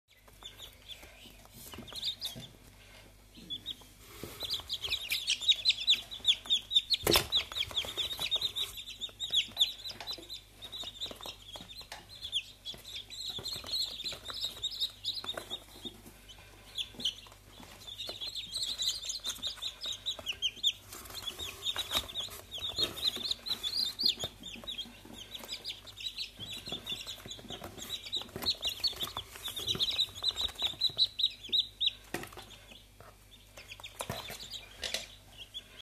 Pui de gaina BRAHMA SILVER ORANGE
Rasa brahma este de origine asiatica , zona Indiana Brahma Putra . In Europa a fost importata din America in secolul al XIX-lea. Este o rasa masiva, rustica ,cu temperament limfatic si cu o infatisare impunatoare.Este importanta ca material ameliator datorita rusticitatii sale si puterii de aclimatizare , calitatii superioare a carnii ,intensitatii ouatului in lunile de iarna,puterii de asimilare si aptitudinii de ingrasare.
Varietati de culoare : herminat deschis , herminat inchis , potarnichie , albastra,aurie .
Productie oua : 120-140 oua/an cu greutate medie de 60 gr.
Greutate corporala : cocos – 4.5-5.5 kg , gaina 3.5-4.5 kg